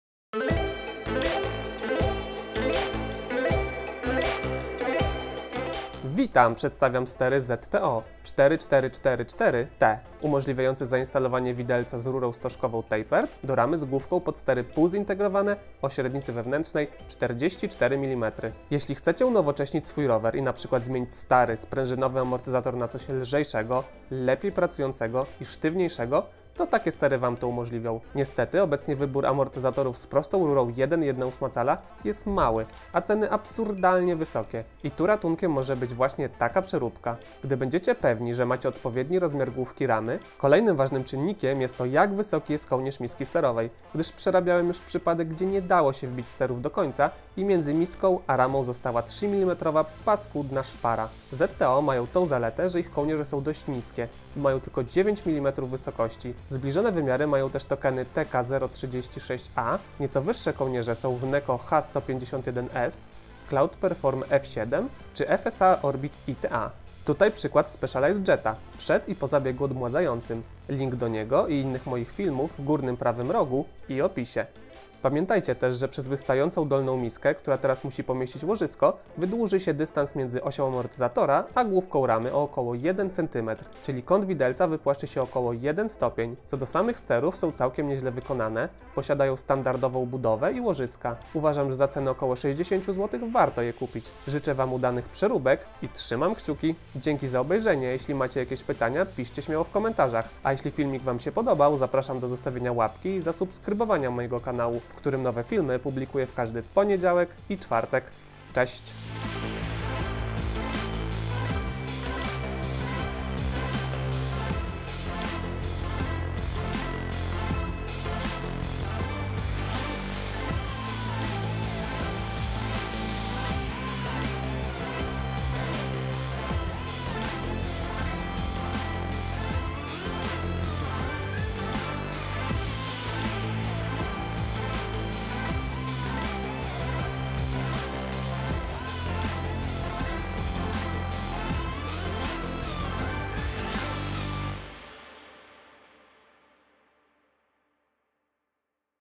STERY ZTTO 4444T // JAK ZAMONTOWAĆ AMORTYZATOR Z RURĄ STOŻKOWĄ TAPERED DO RAMY Z GŁÓWKA 44mm
Zapraszam na film, w którym pokaże zmianę sterów półzintegrowanych,  ZTTO 4444T umożliwiające montaż widelców z rurą stożkową Tapered.

0:00 Wstęp
0:18 Opis
0:40 RODZAJE GŁÓWEK I STERÓW !!!
0:45 Wysokość kołnierzy misek sterowych
0:56 Alternatywne stery
1:07 Stery na rowerze
1:14 Konsekwencje montażu takich sterów
1:26 Jakość wykonania
1:31 Cena
1:36 Budowa sterów